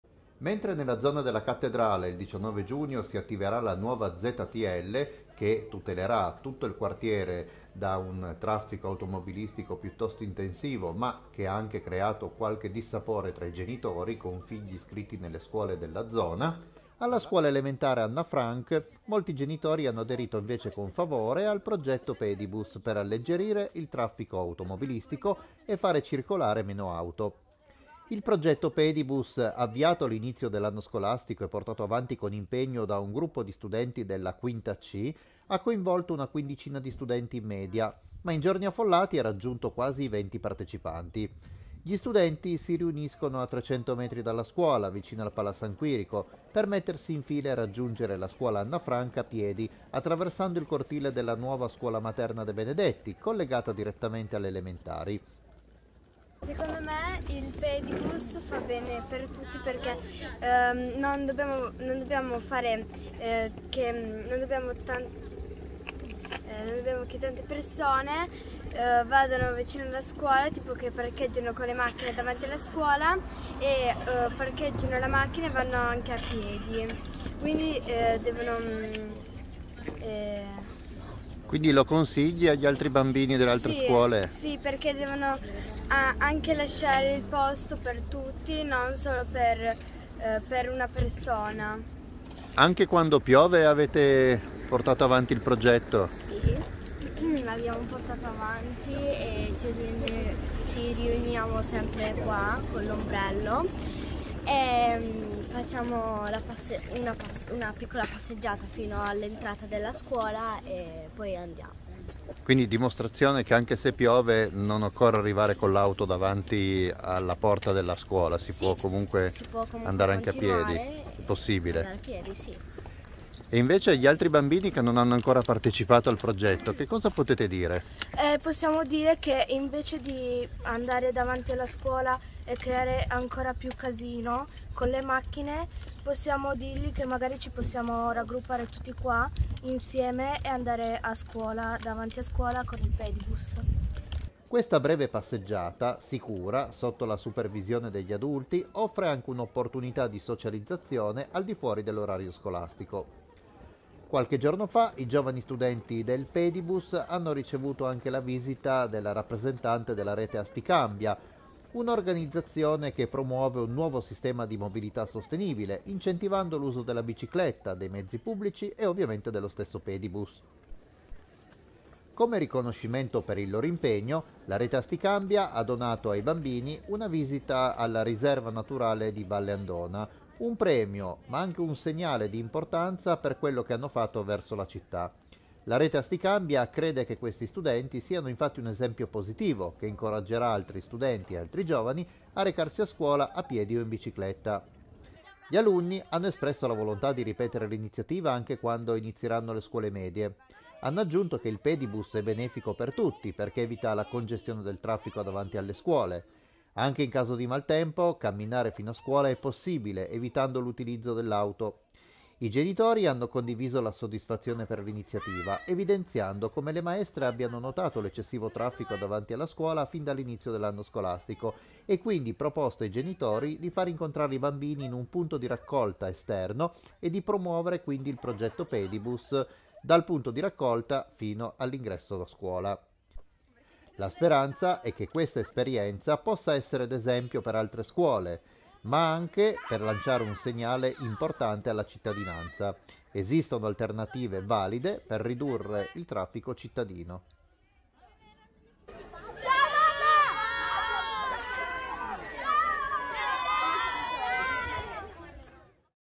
Alla scuola Anna Frank di Asti un anno di "pedibus" per gli alunni della 5°C
Con la chiusura dell'anno scolastico, gli alunni della classe 5°C della scuola elementare Anna Frank fanno un bilancio del progetto "pedibus" voluto per tenere lontane le auto dall'ingresso dell'istituto.

Un bilancio più che positivo perché andare a piedi a scuola ha permesso loro di aiutare l'ambiente, evitare di creare traffico davanti alla scuola e di socializzare fuori dall'orario scolastico.

Un'iniziativa, quella del pedibus alla Anna Frank, sostenuta anche dalle rete Asti Cambia che ha voluto fanno un regalo a tutti i partecipanti.

Videoservizio di Riccardo Santagati